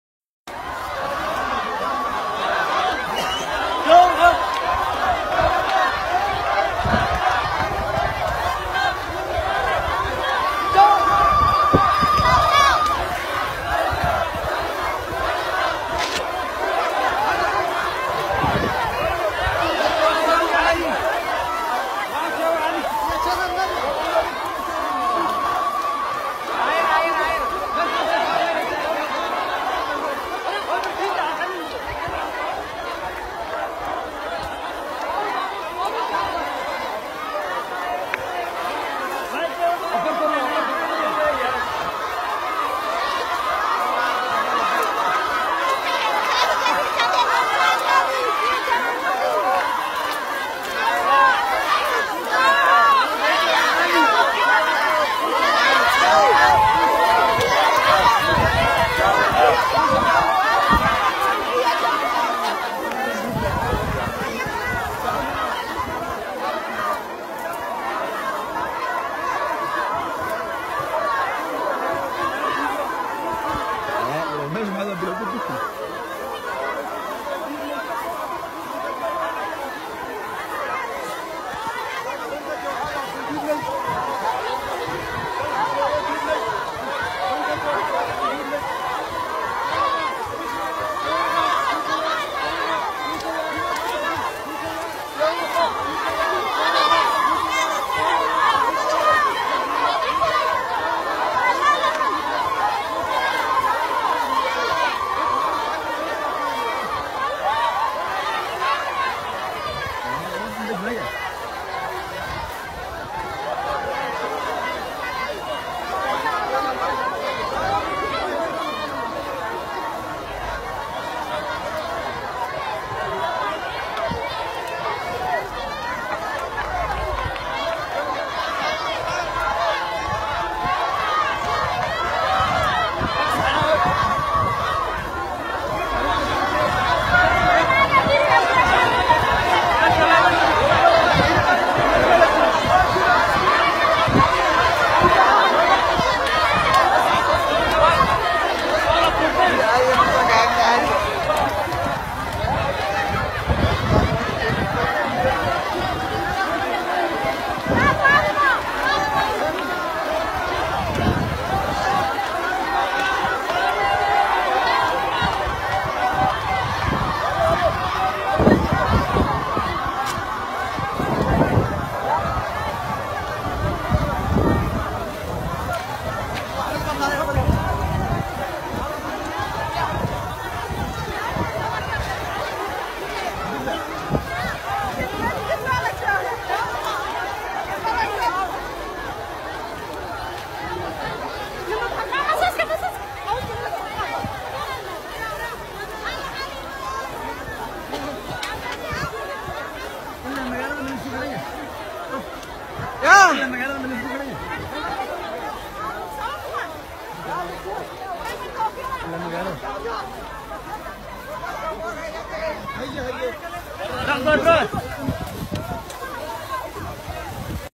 ONLF to Jijiga
A battalion of ONLF Guerilla fighter arrived at Jijiga Airport where thousands os ONLF supporters cheering for a warm welcome.